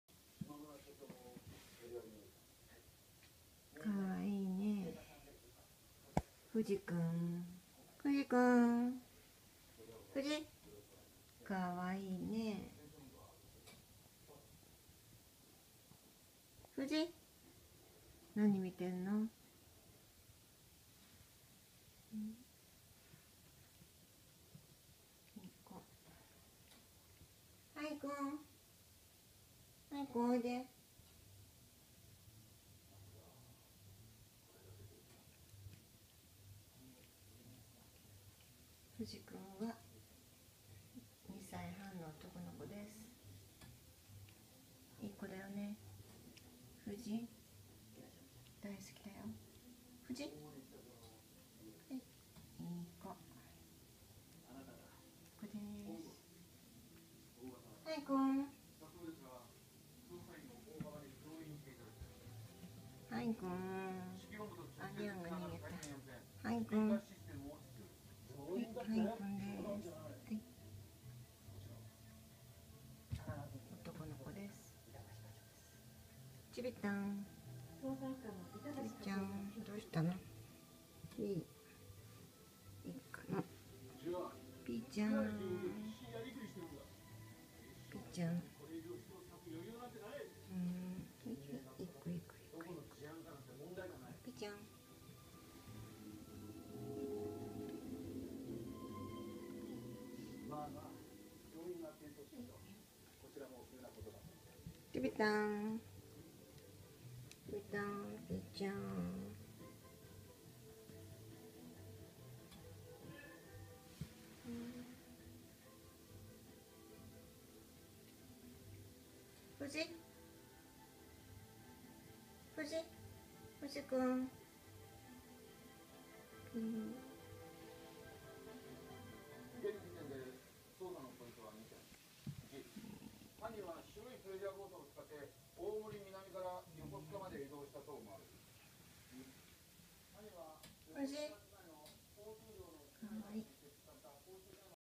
フジ君と仲間たち！
うちの可愛い猫たちです！ずっと一緒にいようね。